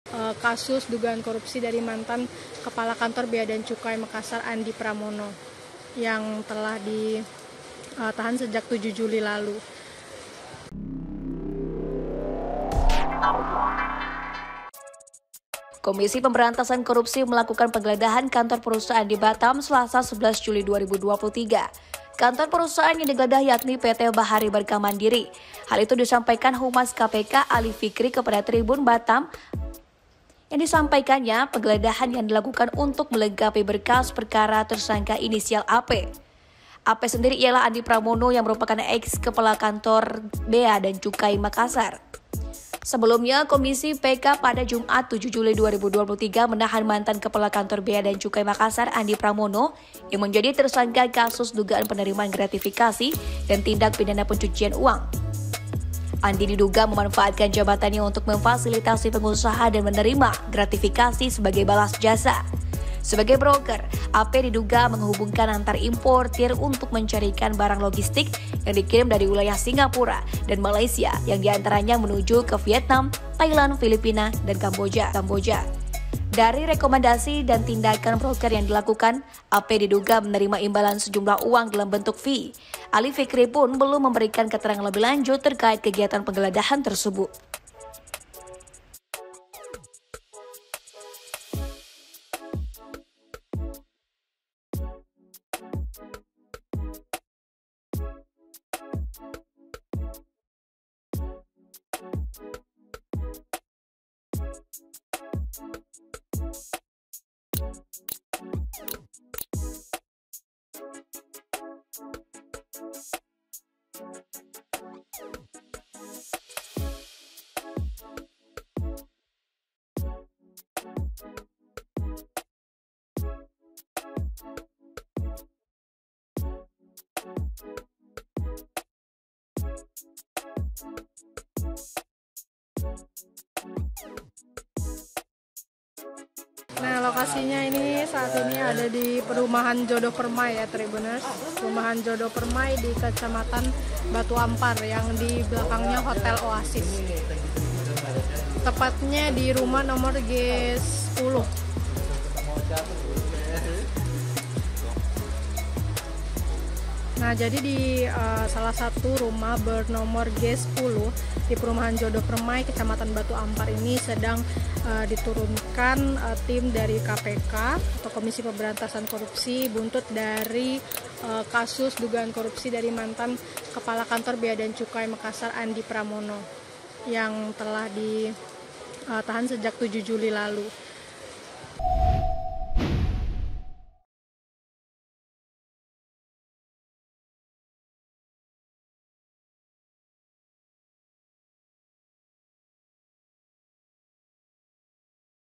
KPK Geledah Kantor PT Bahari Berkah Mandiri Batam untuk Lengkapi Berkas Kasus Andi Pramono
Komisi Pemberantasan Korupsi (KPK) melakukan penggeledahan kantor perusahaan di Batam, Selasa (11/7/2023).

Kantor perusahaan yang digeledah yakni PT. PT Bahari Berkah Mandiri.

Hal itu disampaikan Humas KPK, Ali Fikri kepada Tribunbatam.id melalui pesan WhatsApp.

“Tim penyidik KPK melakukan penggeledahan kantor PT BBM ( Bahari Berkah Mandiri) di Wilayah Batam,” sebutnya.

Disampaikannya, penggeledahan yang dilakukan untuk melengkapi berkas perkara tersangka inisial AP.

AP sendiri ialah Andhi Pramono yang merupakan eks Kepala Kantor Bea dan Cukai Makassar.

Sebelumnya, Komisi PK pada Jumat (7/7/2023) menahan mantan Kepala Kantor Bea dan Cukai Makassar Andhi Pramono yang menjadi tersangka kasus dugaan penerimaan gratifikasi dan tindak pidana pencucian uang (TPPU).

VE: d2k
VO: Mega